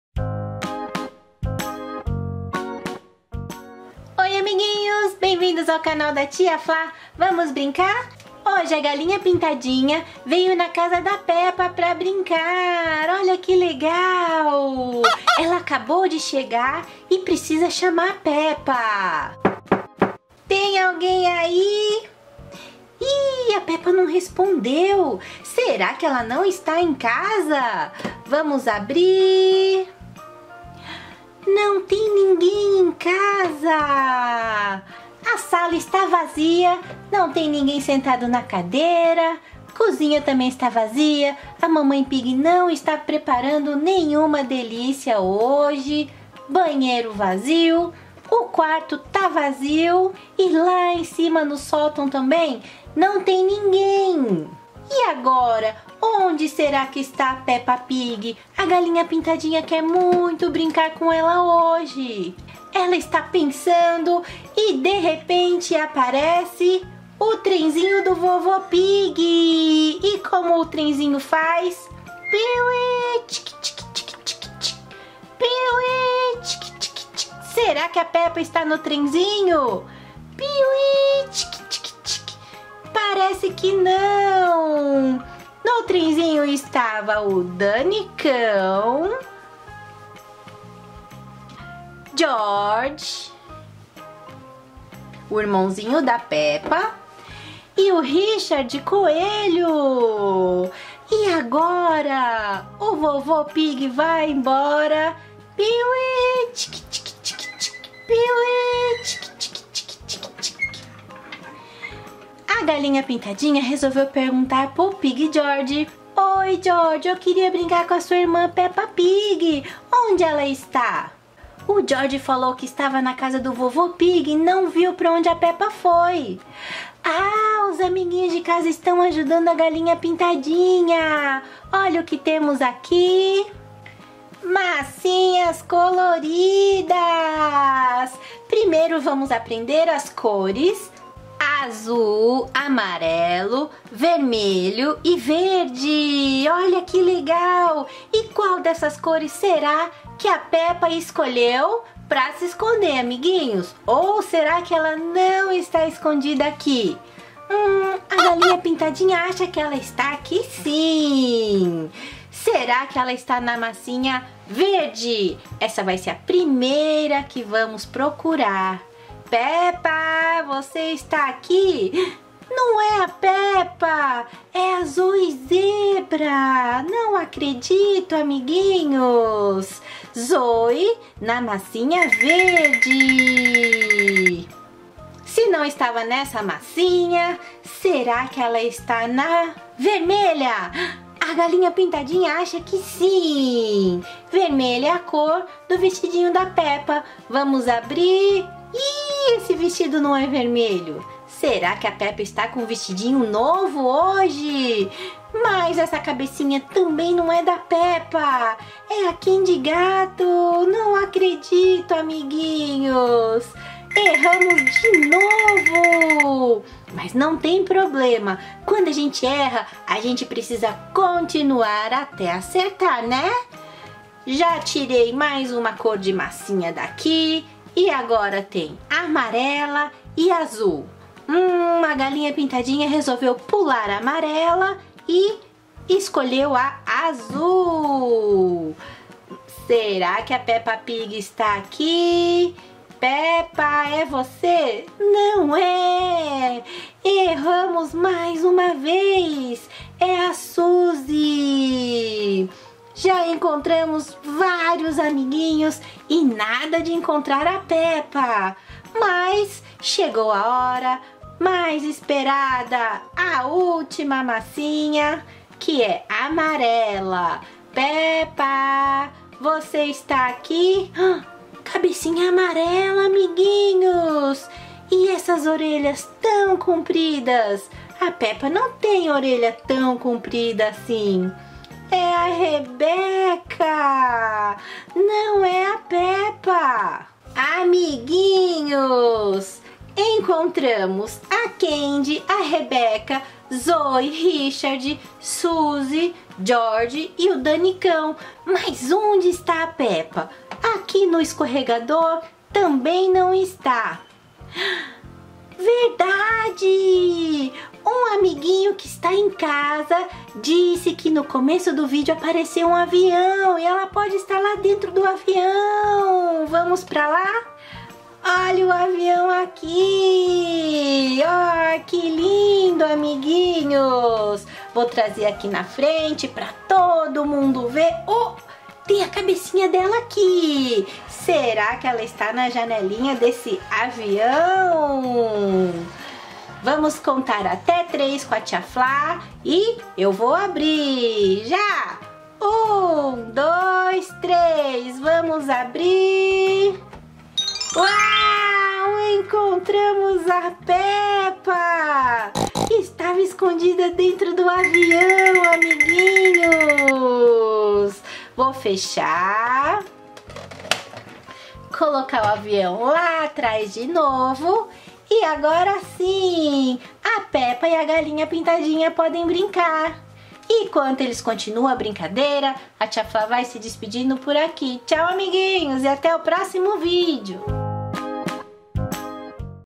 Peppa Pig Sumiu! Galinha Pintadinha Sai Para Procurá-la!
Peppa Pig Sumiu? Não... ela está brincando com a Galinha Pintadinha!